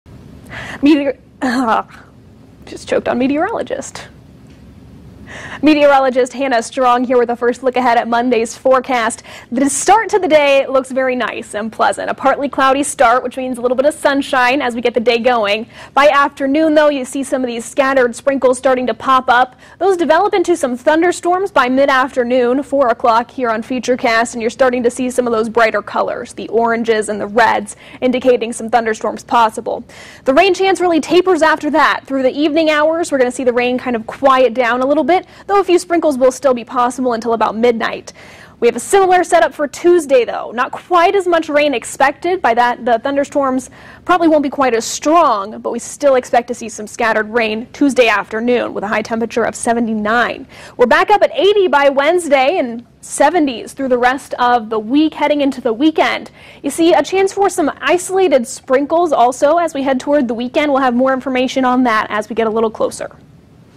Monday Preview
A pleasant start to Monday with PM rain/storms.